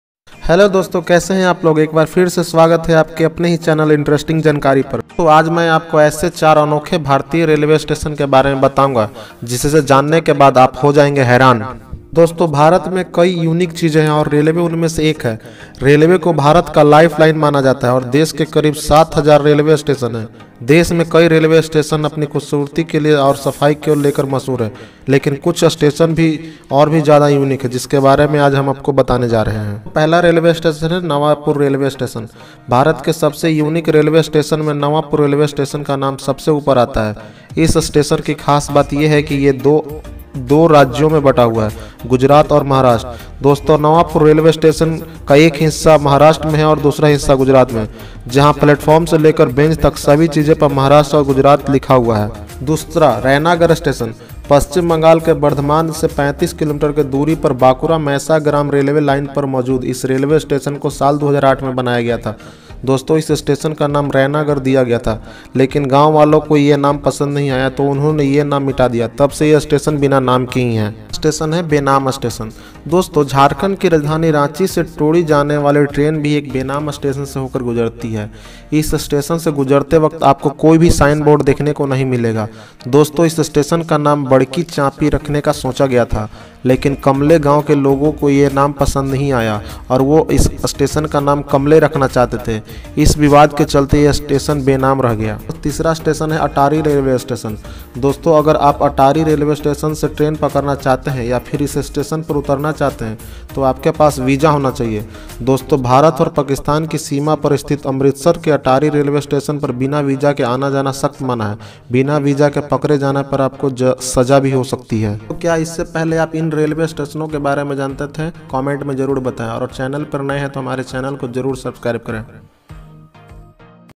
shortsतीन अनोखे😱😱 भारतीय रेलवे स्टेशन।जान कर हो जाएंगे हैरान।जरूर देखें।
Social account:-